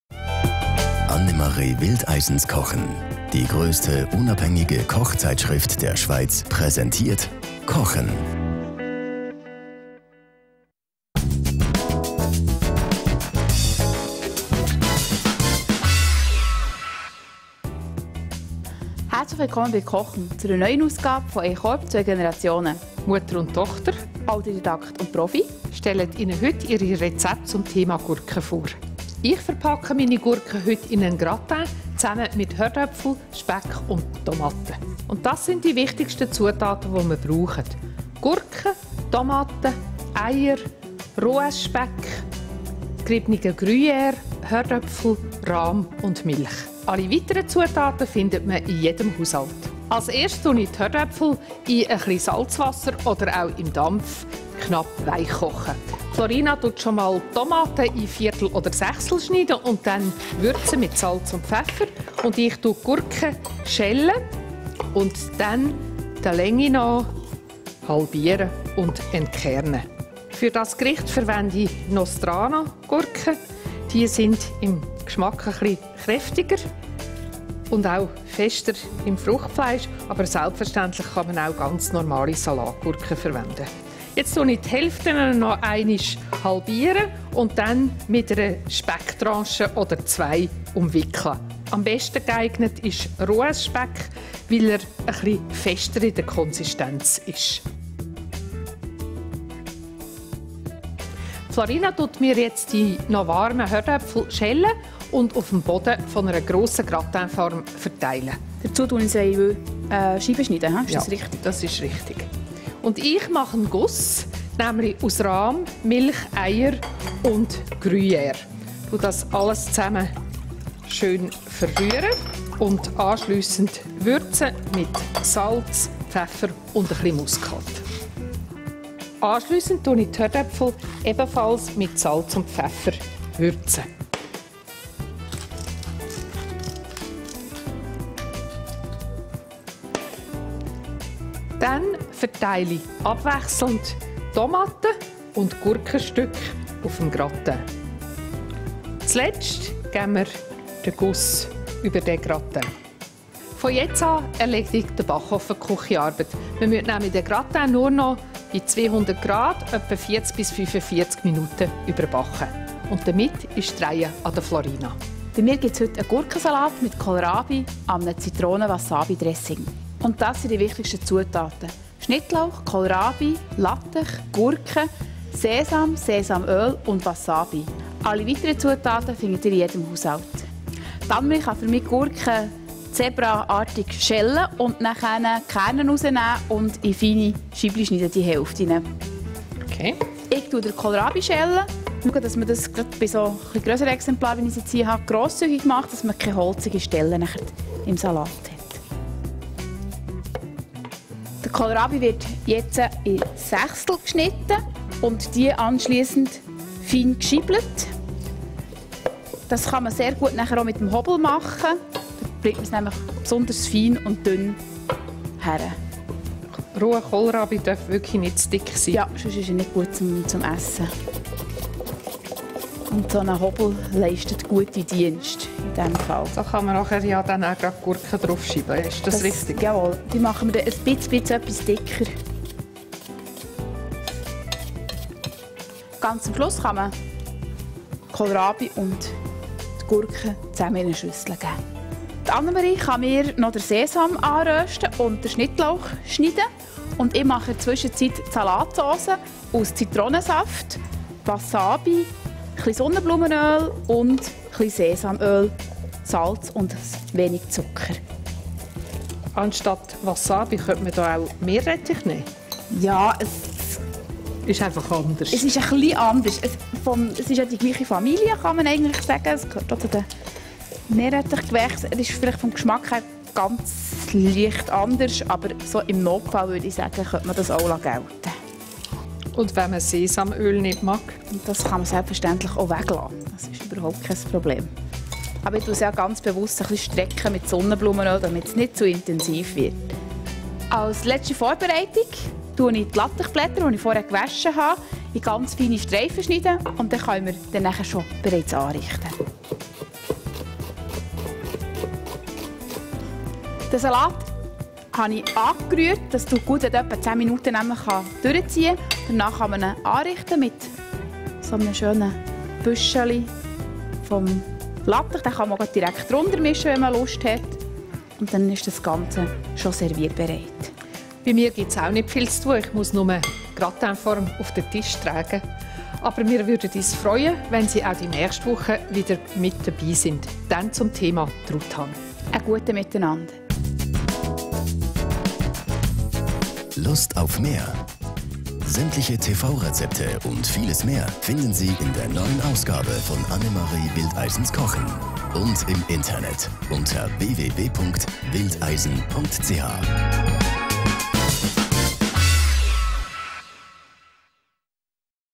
Überbackene Gurken mit Tomaten und Kartoffeln | Gurken-Kohlrabi-Salat an Wasabisauce
Überbackene Gurken mit Tomaten und Kartoffeln: Der Rohess- oder Bauernspeck gibt diesem Gratin eine herzhafte Würze. Und die Gurken präsentieren warm eine ganz neue Seite von sich.

Gurken-Kohlrabi-Salat an Wasabisauce: Die Wasabisauce sorgt in diesem Salat nicht nur für eine schöne Schärfe, sondern auch für einen wunderbar asiatischen Touch.